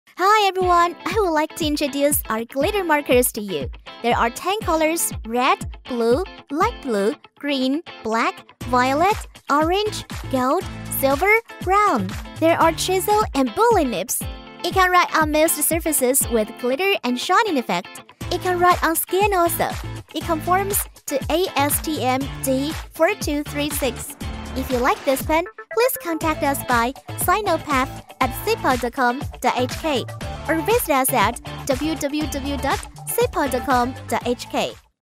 SPG006 - Glitter Markers
Our line of glitter markers come with many nib sizes as well as a wide range of colors. Perfect for arts, crafts, and designs.